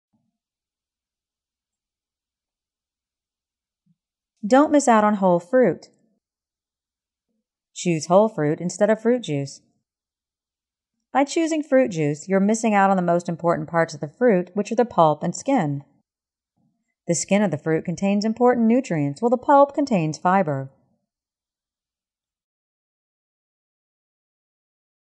Don't Miss Out on Whole Fruit
Choose whole fruit instead of fruit juice.
By choosing fruit juice, you are missing out on the most important parts of the fruit which are the pulp and skin.
The skin of the fruit contains important nutrients while the pulp contains fiber.